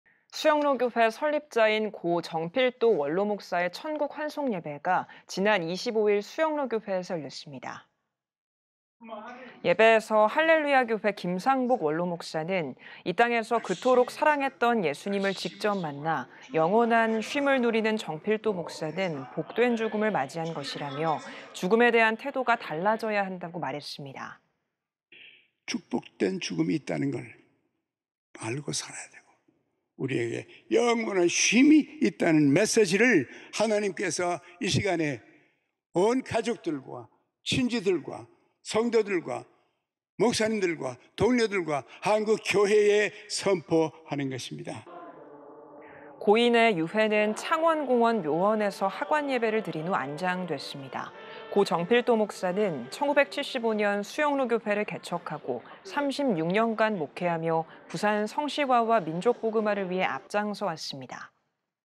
故 정필도 목사 천국 환송예배 (부산, 박주은) l CTS뉴스
수영로교회 설립자인 故 정필도 원로목사의 천국환송예배가 25일 수영로교회에서 열렸습니다.

예배에서 할렐루야교회 김상복 원로목사는 "이 땅에서 그토록 사랑했던 예수님을 직접 만나 영원한 쉼을 누리는 정필도 목사는  복된 죽음을 맞은 것"이라며 "죽음에 대한 태도가 달라져야한다"고 말했습니다.

김상복 목사 / 할렐루야교회 원로
축복된 죽음이 있다는 것을 알고 살아야되고 우리에게 영원한 쉼이 있다는 메시지를 하나님께서 이 시간에 온 가족들과 친지들과 성도들과 목사님들과 동료들과 한국교회에 선포하는 것입니다.

고인의 유해는 창원공원묘원에서 하관예배를 드린 후 안장됐습니다.
故 정필도 목사는 1975년 수영로교회를 개척하고 36년간 목회하며 부산 성시화와 민족 복음화를 위해 앞장서 왔습니다.